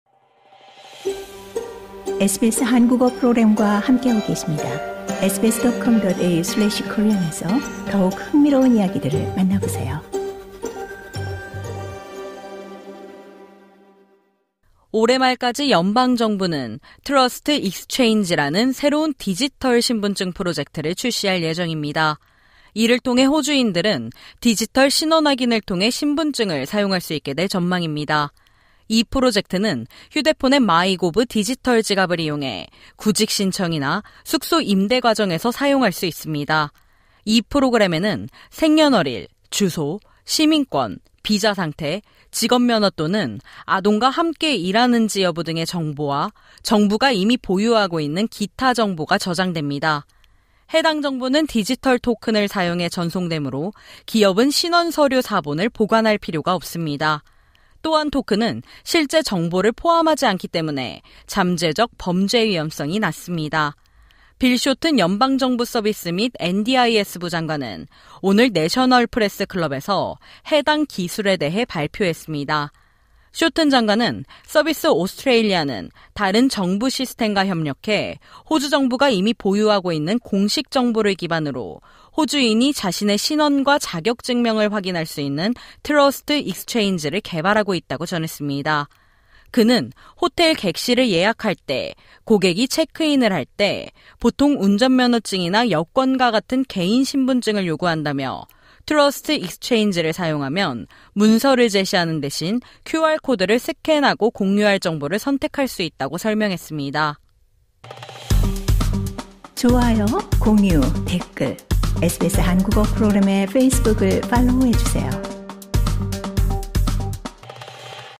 'QR코드'로 구직 신청부터 숙소 임대까지…디지털 신분증 프로젝트 출시 예정 | SBS Korean - SBS 한국어 프로그램
호주 연방 정부는 올해 말 '트러스트 익스체인지(Trust Exchange, TEx)'라는 새로운 디지털 신분증 프로젝트를 출시할 예정입니다. 이에 따라 QR코드가 신분증을 대체할 수 있을 것으로 전망됩니다.